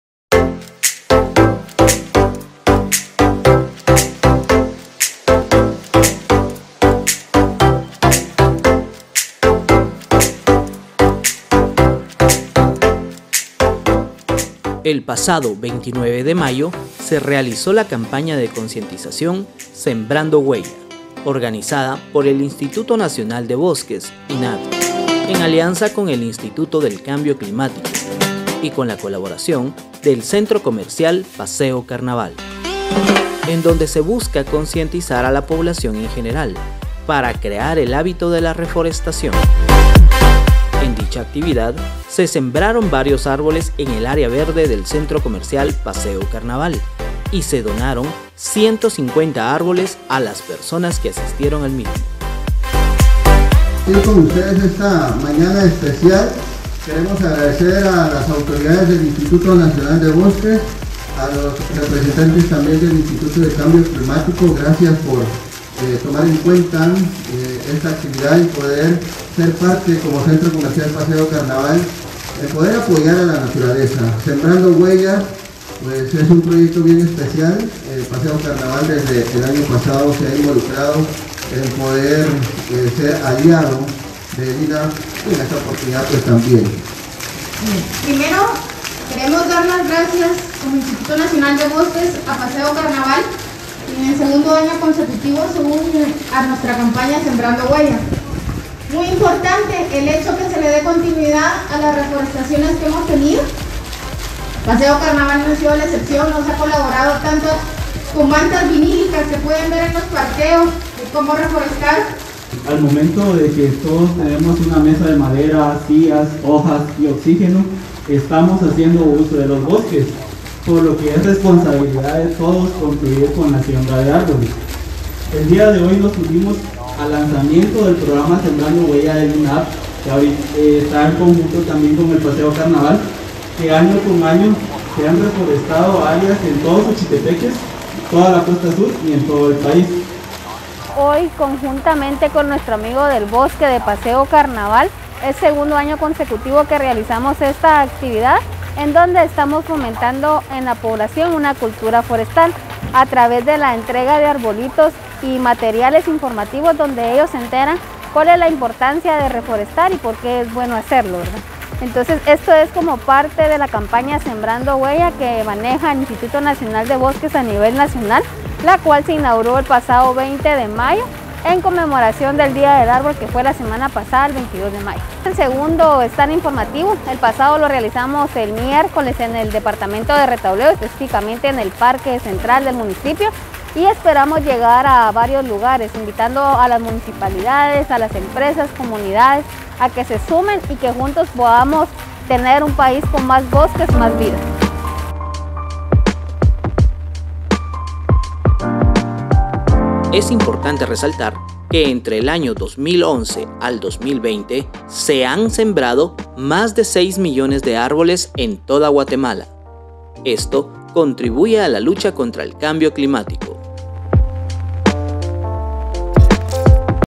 CAMPAÑA SEMBRANDO HUELLA - INAB - ICC - CC PASEO CARNAVAL - mazatecos.com
El pasado 29 de mayo se realizó la campaña de concientización “Sembrando Huella” organizada por el Instituto Nacional de Bosques –
INAB Guatemala, en alianza con el ICC - Instituto Privado de Investigación sobre Cambio Climático y con la colaboración de Paseo Carnaval - Centro Comercial

En donde se busca concientizar a la población en general, para crear el hábito de la reforestación.

En dicha actividad se sembraron varios árboles en el área verde de C.C. Paseo Carnaval y se donaron 150 árboles a las personas que asistieron al mismo.

Es importante resaltar que entre el año 2011 al 2020 se han sembrado más de 6 millones de árboles en toda Guatemala, esto contribuye a la lucha contra el cambio climático.

Cledia Alvizures / Fernanda Pérez / Jac Castro / Josselline Gomez / Miss Suchitepequez Latina / Keops Delkairo / Cristian Icó Soto